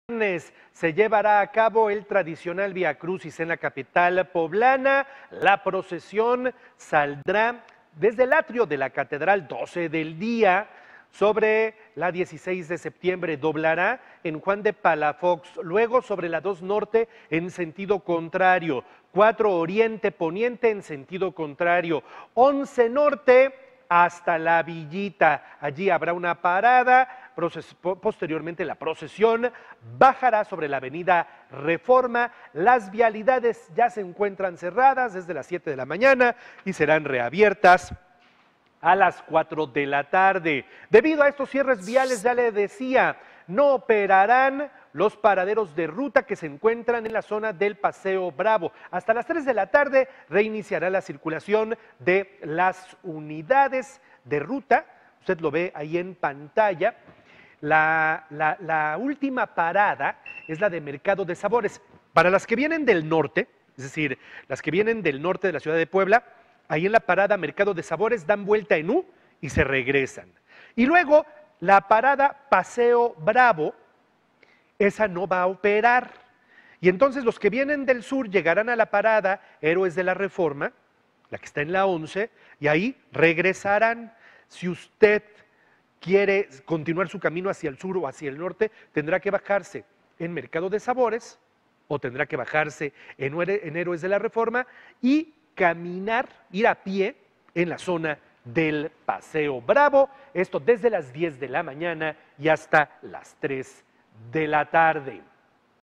Cierres y RUTA || Noticias con Juan Carlos Valerio
¡Recuerde tomar sus precauciones! Hay cierres viales y modificaciones en la línea 2 de RUTA.